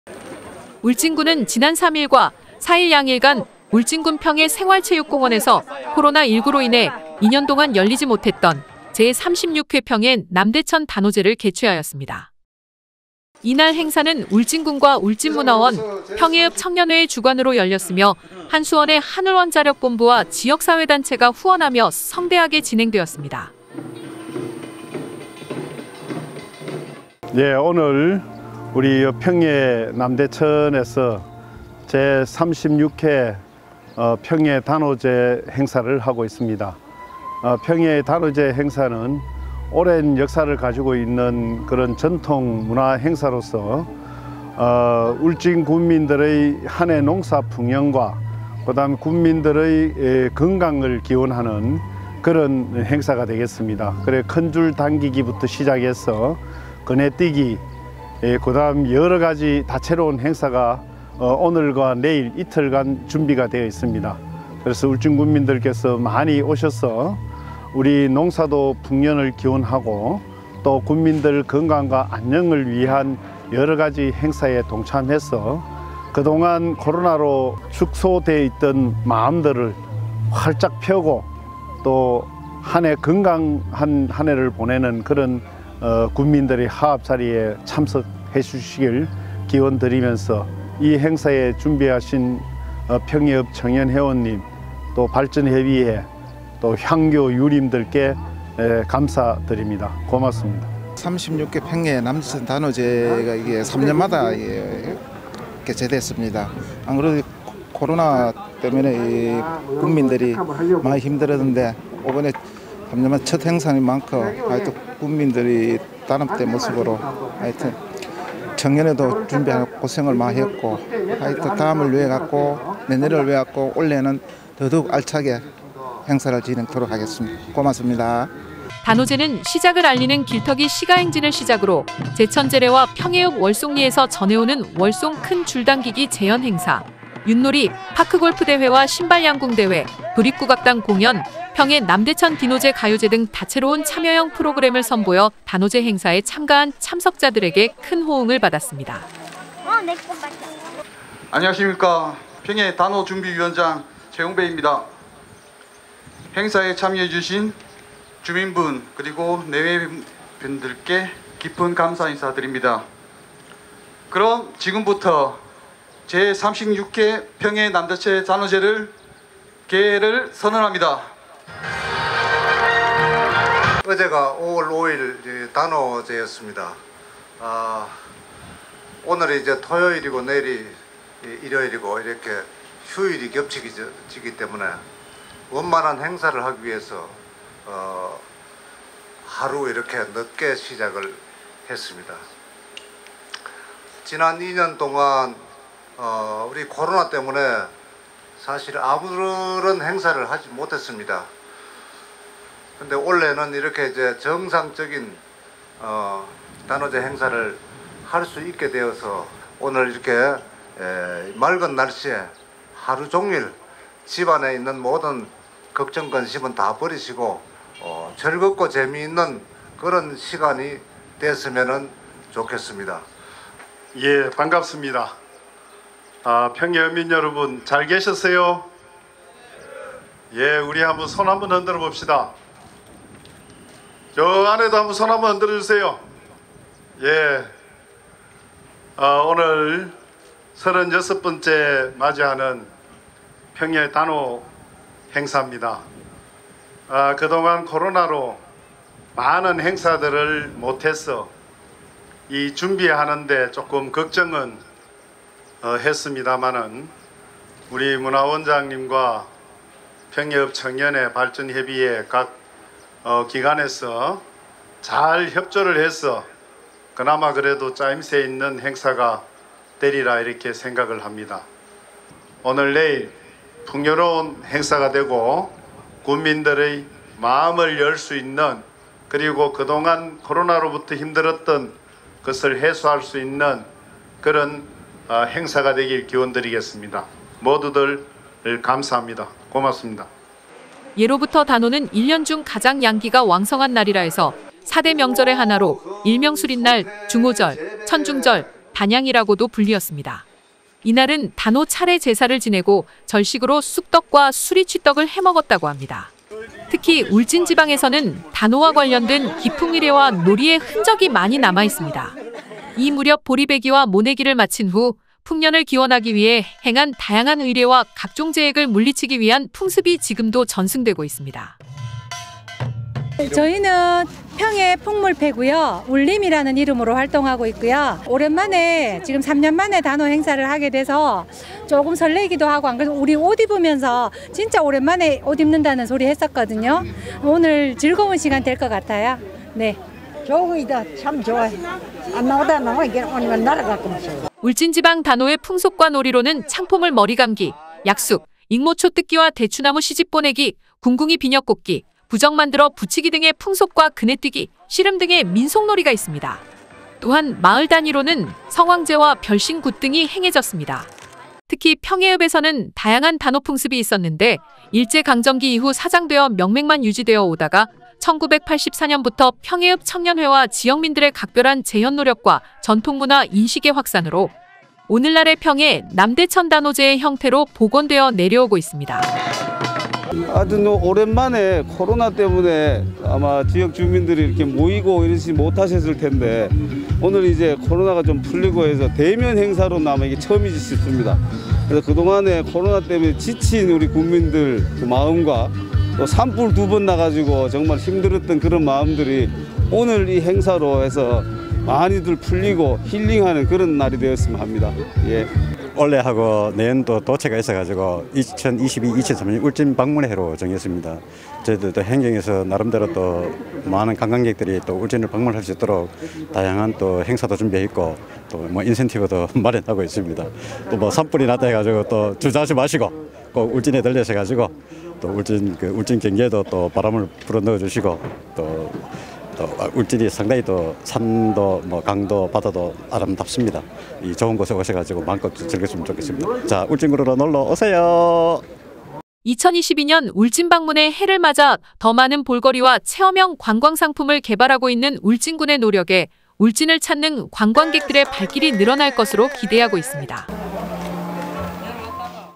[문화] 2년만에 다시 열린 제 36회 울진 '평해 남대천 단오제'
20220603(토)
(기자)
울진군은 지난 3일과 4일 양일간 울진군 평해읍 평해생활체육공원에서 코로나19로 인해 2년동안 열리지 못했던 제 36회 평해 남대천 단오제를 개최하였습니다.

이날 행사는 울진군과 울진문화원, 평해읍청년회의 주관으로 열렸으며 한수원의 한울원자력본부와 지역사회단체가 후원하며 성대하게 진행되었습니다.
-인터뷰/ 전찬걸 울진군수
-인터뷰/ 김광욱 평해읍장

단오제는  시작을 알리는 길터기 시가행진을 시작으로,  제천제례, 평해읍
월송리에서 전해오는 월송 큰 줄당기기 재연 행사, 윷놀이, 파크골프대회와
신발양궁대회, 도립국악단 공연, 평해 남대천 가요제 가요제등 다채로운
참여형 프로그램을 선보여   단오제 행사에 참가한  참석자들에게 큰 호응을 받았습니다.

-최용배 단오제준비위원장
-김성준 울진문화원장
-전찬걸 울진군수

예로부터 단오는 일년 중 가장 양기가 왕성한 날이라 해서 4대 명절의 하나로
일명 수릿날, 중오절, 천중절, 단양이라고도 불리었습니다.
이날은 단오 차례 제사를 지내고 절식으로 쑥떡과 수리취떡을 해먹었다고 합니다.

특히 울진지방에서는 단오와 관련된 기풍의례와 놀이의 흔적이 많이 남아있습니다.
이 무렵 보리베기와 모내기를 마친 후 풍년을 기원하기위해 행한 다양한 의례와
각종재액을 물리치기 위한 풍습이 지금도 전승되고 있습니다.

-인터뷰/김난영/ 평해풍물패 ‘울림’

울진지방 단오의 풍속과 놀이로는 창포물 머리감기, 약쑥, 익모초 뜯기,
대추나무 시집보내기, 궁궁이 비녀꽃기, 부적 만들어 붙이기등의 풍속과
그네뛰기, 씨름 등의 민속놀이가 있습니다. 또한 마을 단위로는 성황제와
별신 굿등이 행해졌습니다.

특히 평해에서는 다양한 단오풍습이 있었는데, 일제 강점기 이후 사장되어
명맥만 유지되어 오다가 1984년부터 평해읍청년회와 지역민들의 각별한
재현노력과 전통문화 인식의 확산으로 오늘날의 평해남대천단오제의 형태로
복원되어 내려오고 있습니다.

-인터뷰/박형수 국회의원
-인터뷰/장현용 울진군문화관광과장

2022년 울진방문의 해를 맞아 더 많은 볼거리와 체험형 관광삼품을 개발하고 있는 울진군의 노력에 울진을 찾는 발길이 늘어날 것으로 기대하고 있습니다.